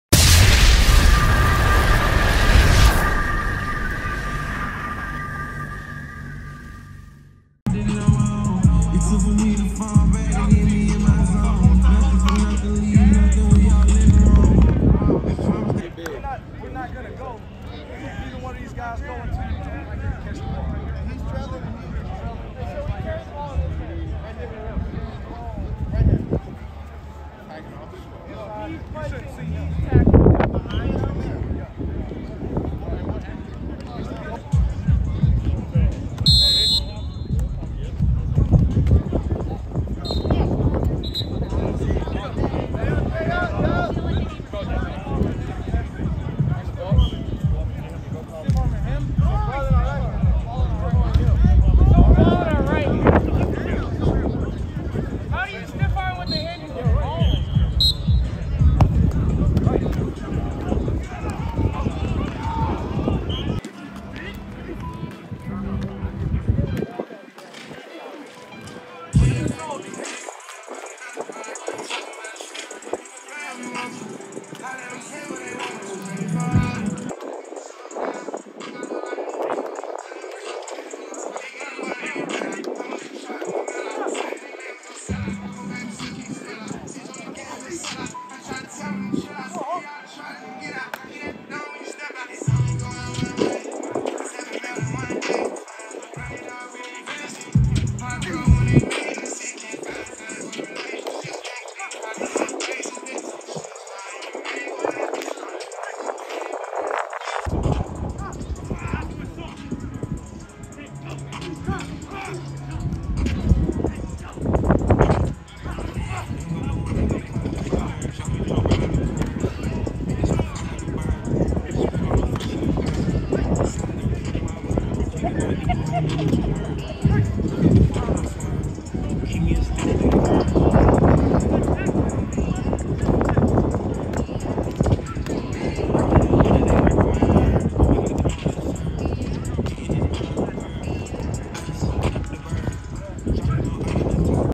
Sights and Sounds from USC's fifth spring practice of 2024
The video begins with Makai Lemon fielding a kick and then the receivers working on ball security drills with the defensive backs. The video ends with defensive back drills.